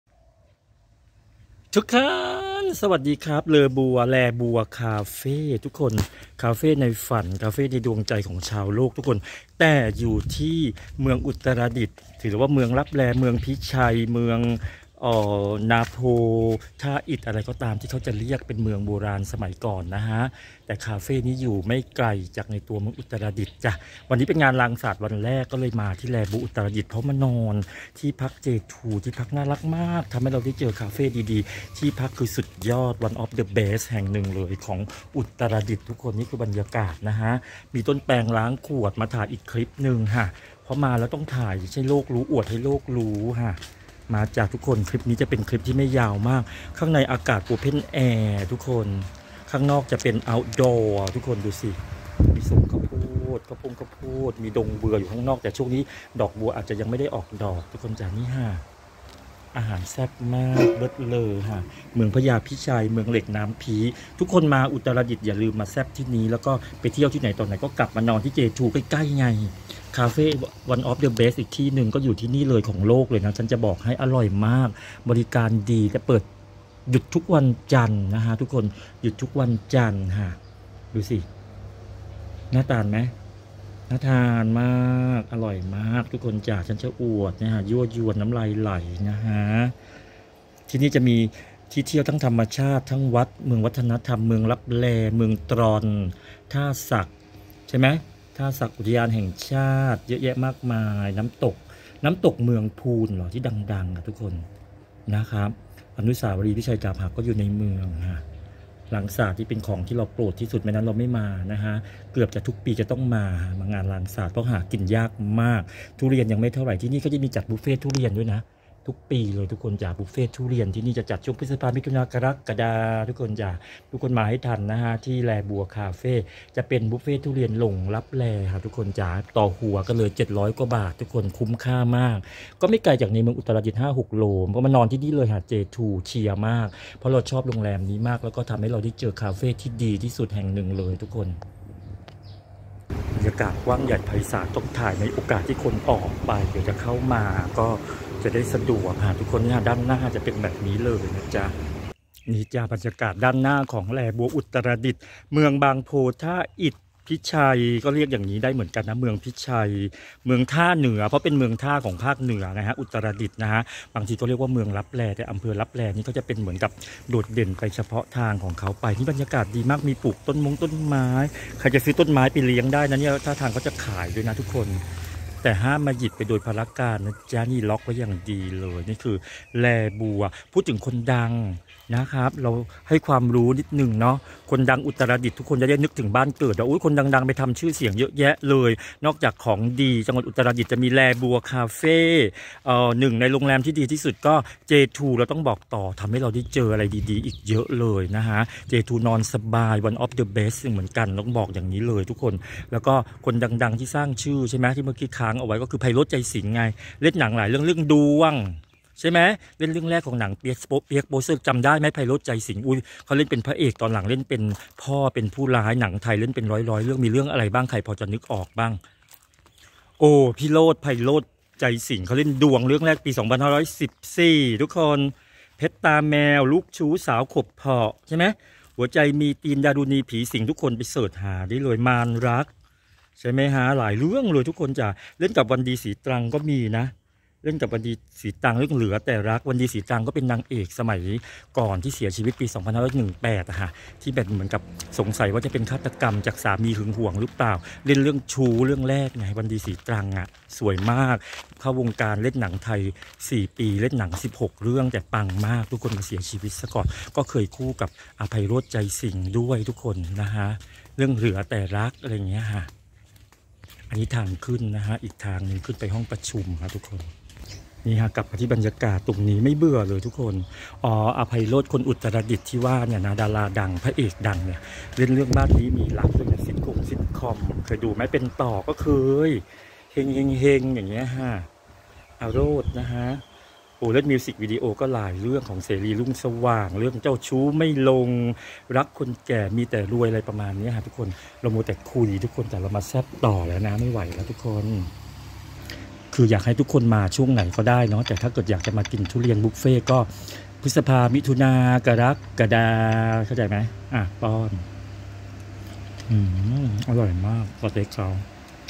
ลางสาดต้องไม่สูญพันธุ์ มาเมืองนี้ต้องไม่พลาดแลบัวคาเฟ่
คลิปนี้พาดหัวคลิปชิวๆสบายๆ
อร่อยมากกกกกกเลิศมากกก

เจอคาเฟ่ที่ปังสุดๆแห่งหนึ่งที่แซบมากๆๆๆต้อง
บอกต่อ ใกล้โรงแรมเจทูที่พักที่ดีที่สุดแห่งหนึ่งในอุตรดิตถ์